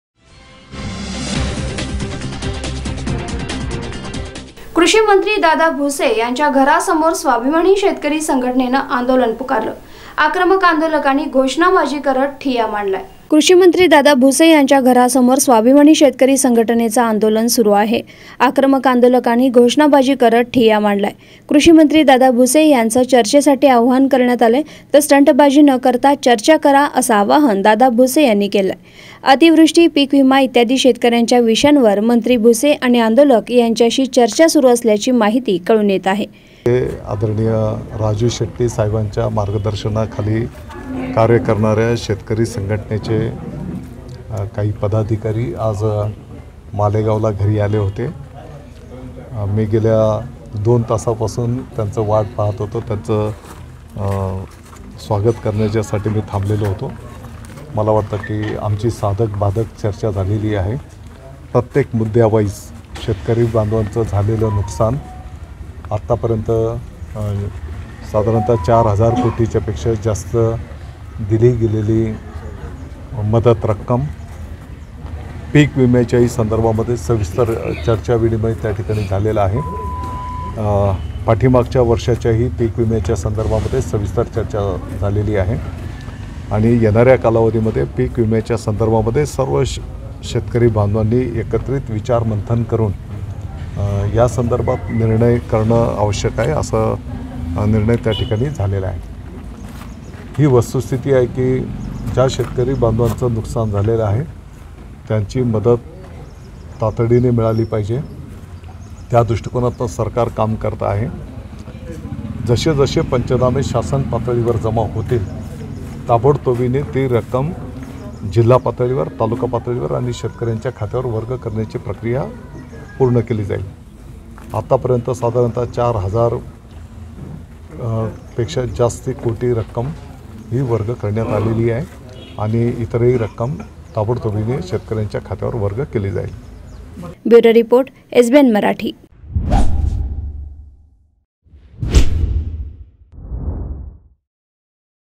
Nashik | कृषिमंत्री | Dada Bhuse | यांच्या घरासमोर स्वाभिमानी शेतकरी संघटनेचे आंदोलन,
कृषिमंत्री दादा भुसे यांच्या घरासमोर स्वाभिमानी शेतकरी संघटनेचे आंदोलन, आक्रमक आंदोलकांनी घोषणाबाजी करत मांडला ठिय्या.@